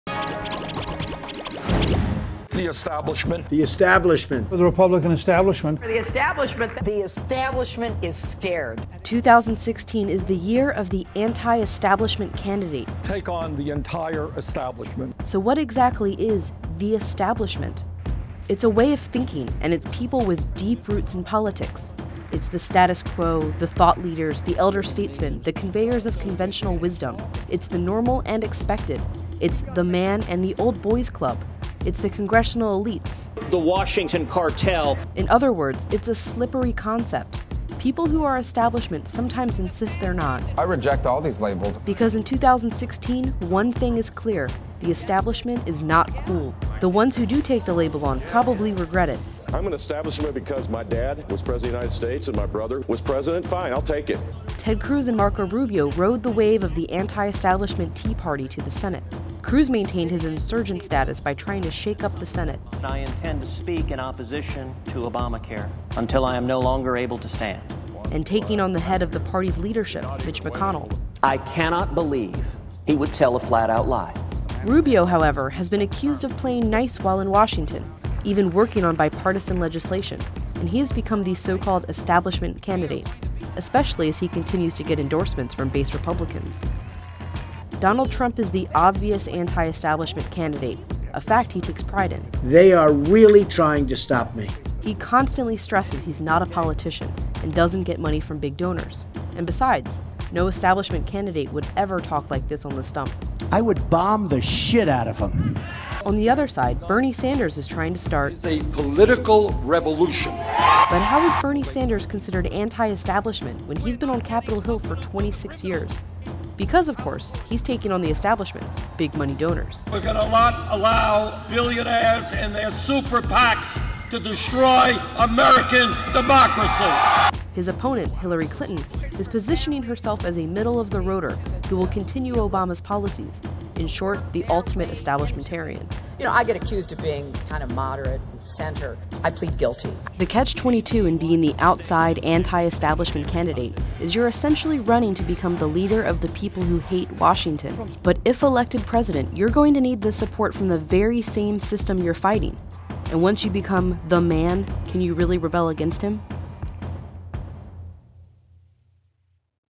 In the 2016 Elections, Who is "The Establishment"?
A common phrase in this election season is "the establishment"—politicians run against it, embrace it or get run over by it. But who's in this supposedly omnipotent group of decision makers? WSJ decodes the phrase on "Campaign Speak." Photo: Getty